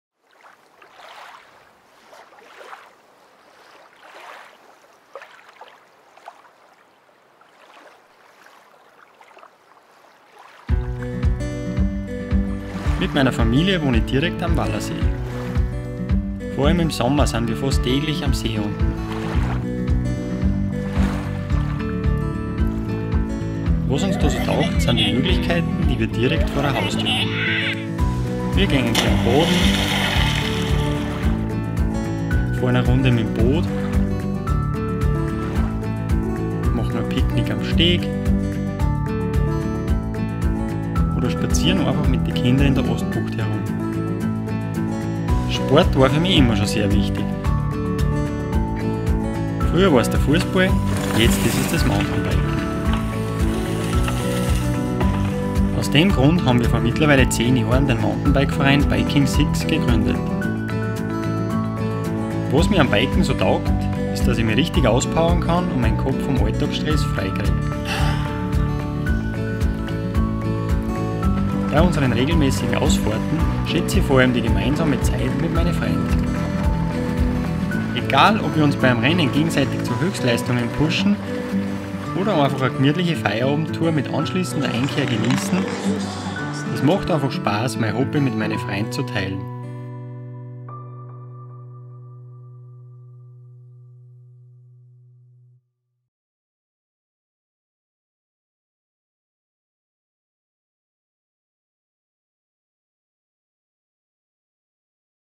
ausgesprochen:lebenswert Christian Schober und sein Leben in der Plusregion
Die Plusregion bietet alles, was man sich für ein erfülltes Leben wünscht. Tolle Jobs und gleichzeitig eine hohe Lebensqualität.

Die 10-teilige Imagekampagne ausgesprochen:lebenswert erzählt echte Geschichten von echten Menschen, die JA zur Region sagen und aufzeigen, warum sie in unserer Region so gerne wohnen, leben, arbeiten, was ihnen wichtig ist und am Herzen liegt.

So auch Christian Schober: Er genießt die Zeit mit seiner Familie am Wallersee - auch Sport ist für ihn sehr wichtig. Seit ca. 10 Jahren ist er Mitglied im Mountainbike-Verein Biking 6 wie es ihm dabei geht, sehr ihr im Video.